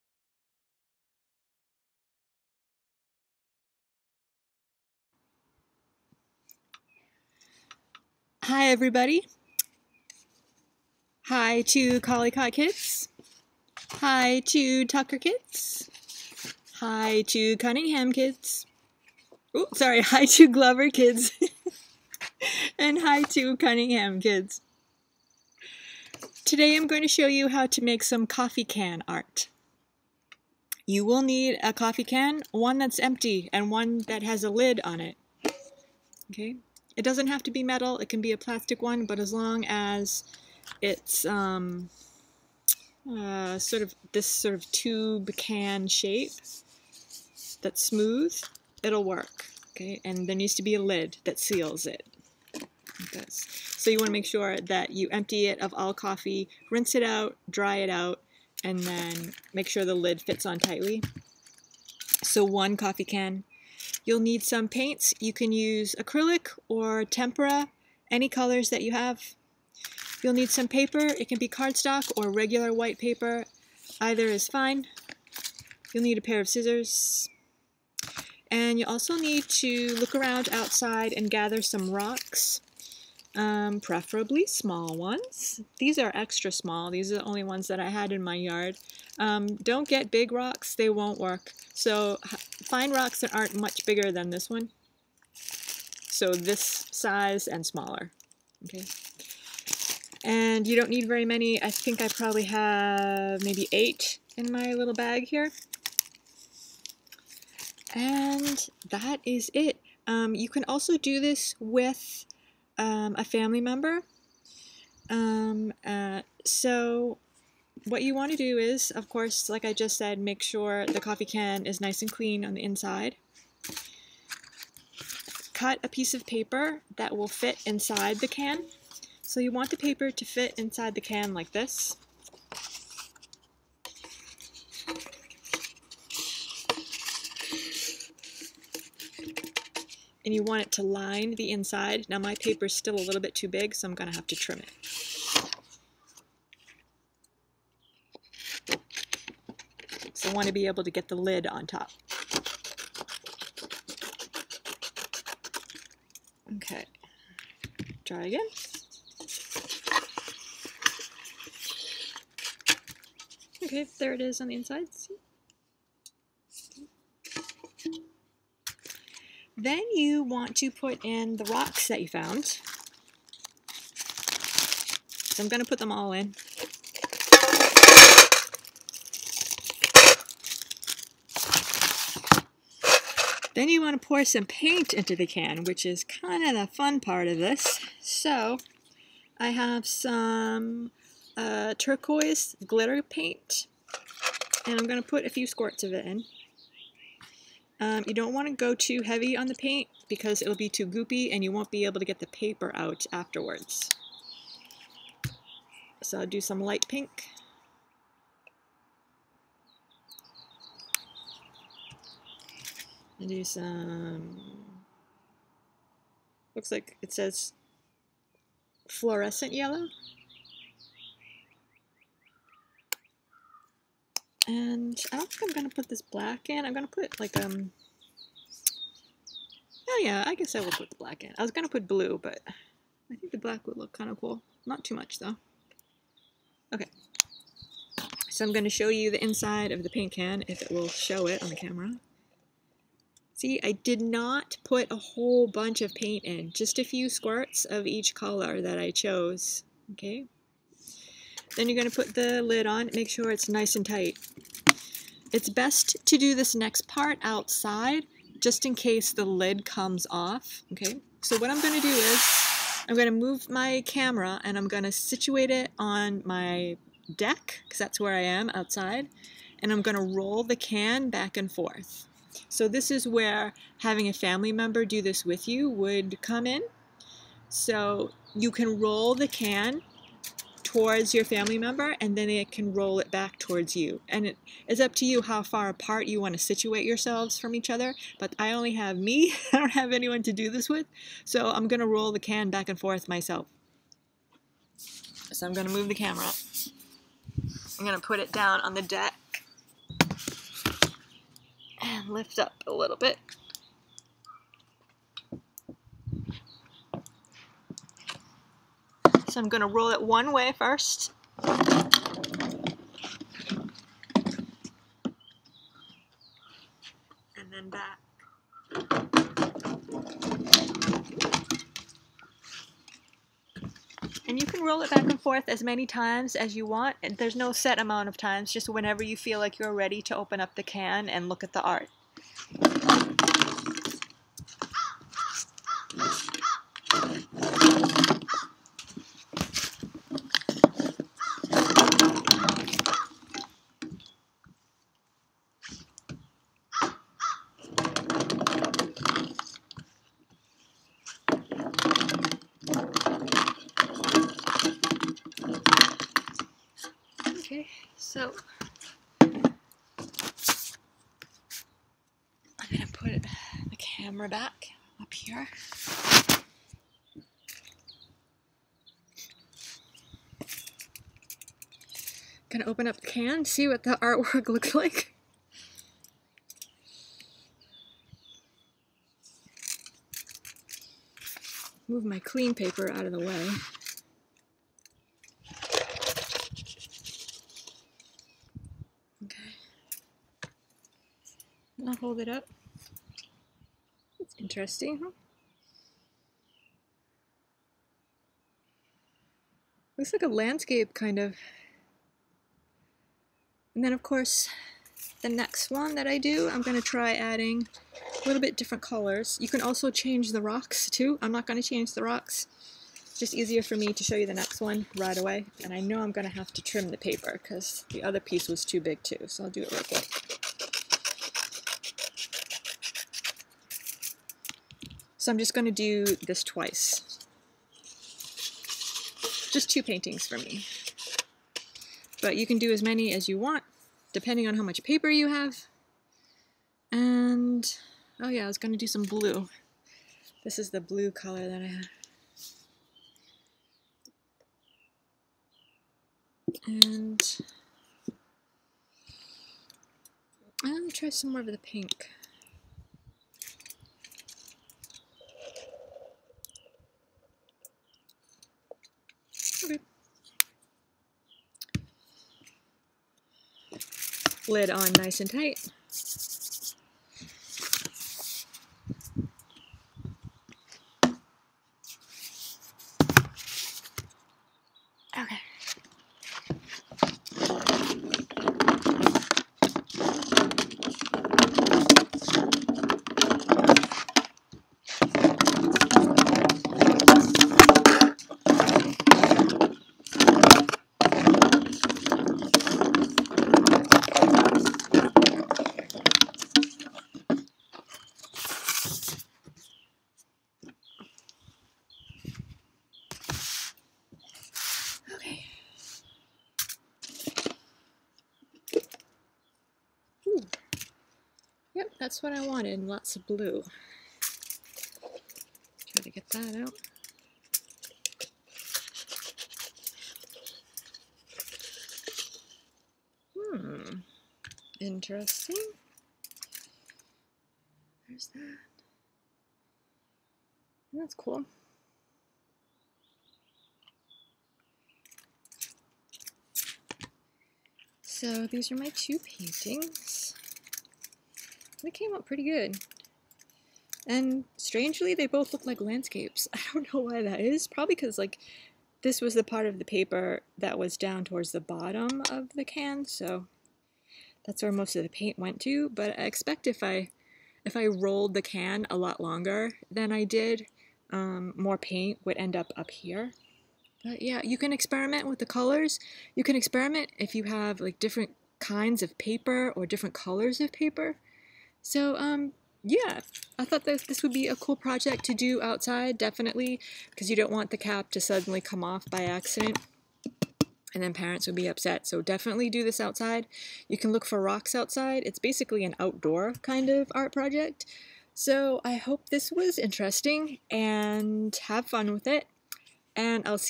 Coffee Can Art Tutorial
How to make art with a coffee can.
I'm Miss Susan from the GASE Glover After school program.